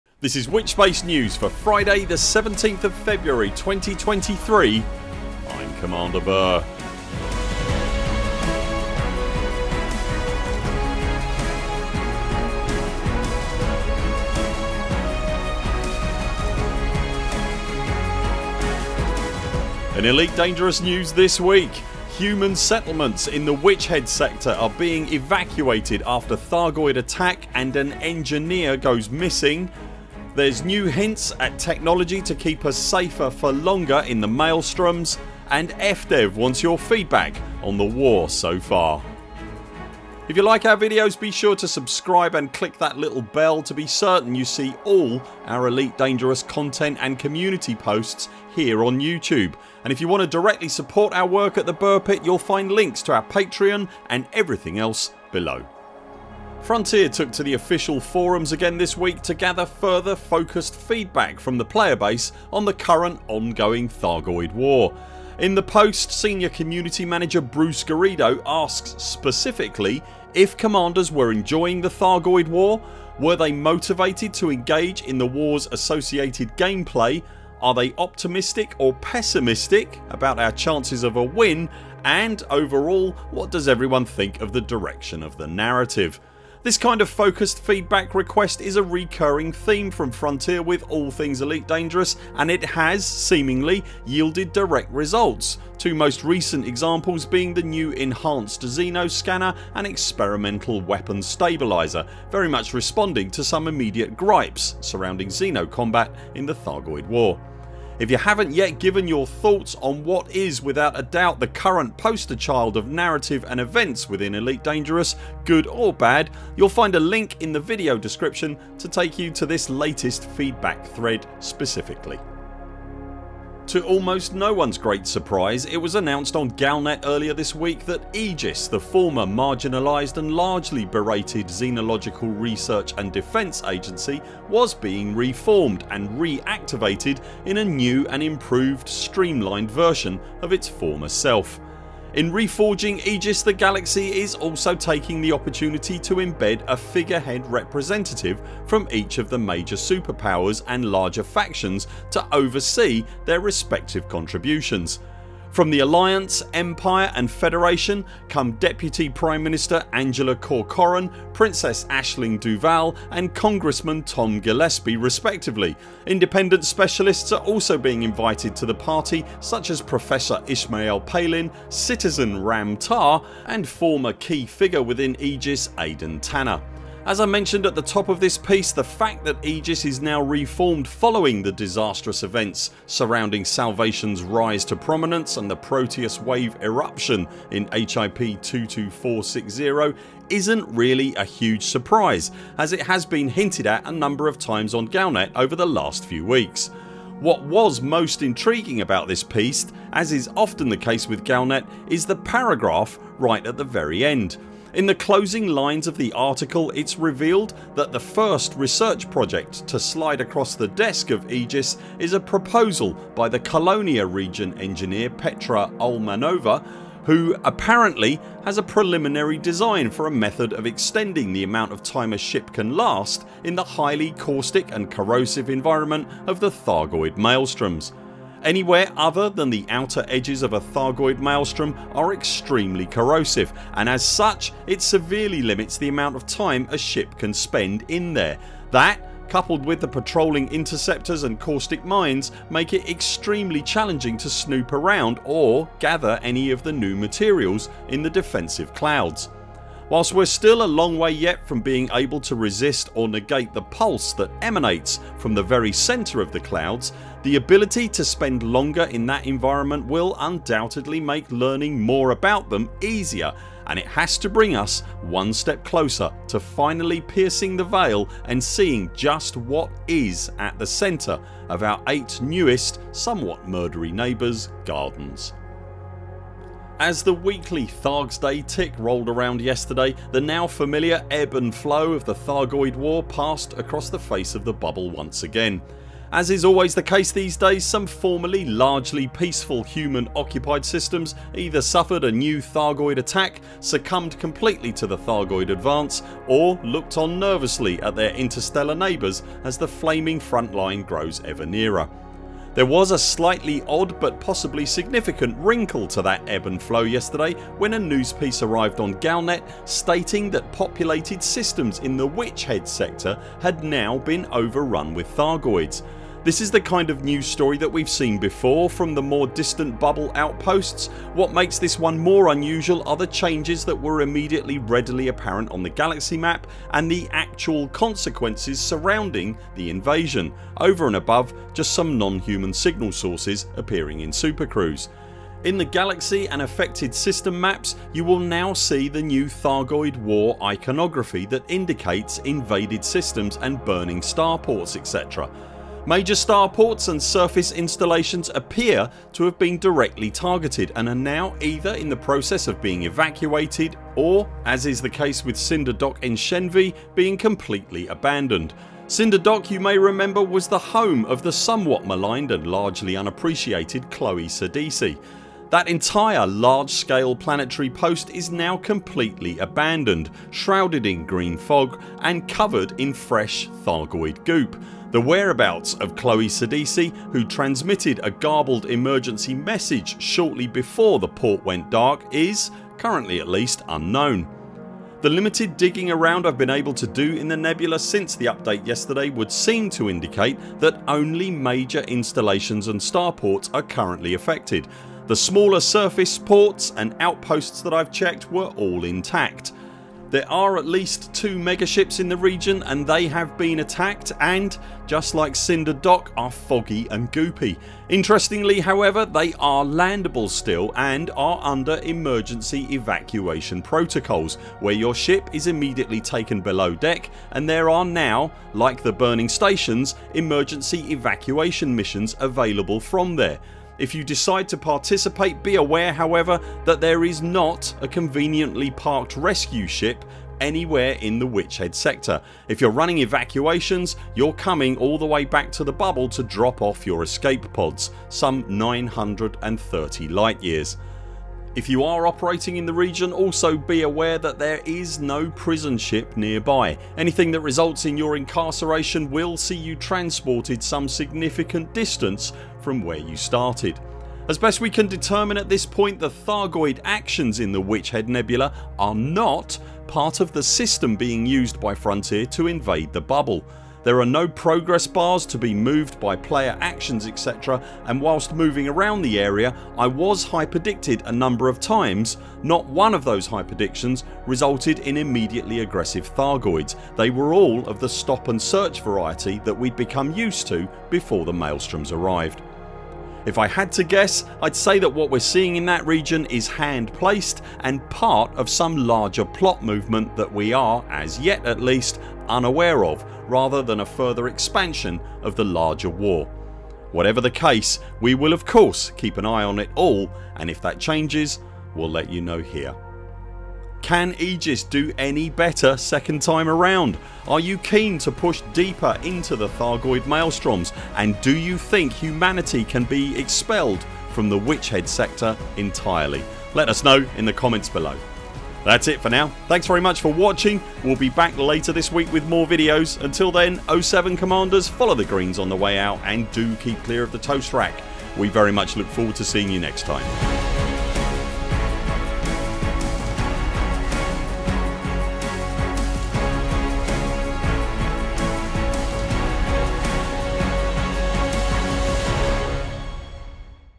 Elite Dangerous News: Engineer Goes Missing, New Maelstrom Defences, Attack on Witch Head Nebula
In Elite Dangerous News this week… Human settlements in the Witch Head Sector are being evacuated after Thargoid attack and an engineer goes missing. There’s new hints at a technology to keep us safer for longer in the Maelstroms. And FDEV wants your feedback on the war so far… o7 CMDRs

Useful Links!

Ian Doncasters Analysis of the Thargoid War Simulation System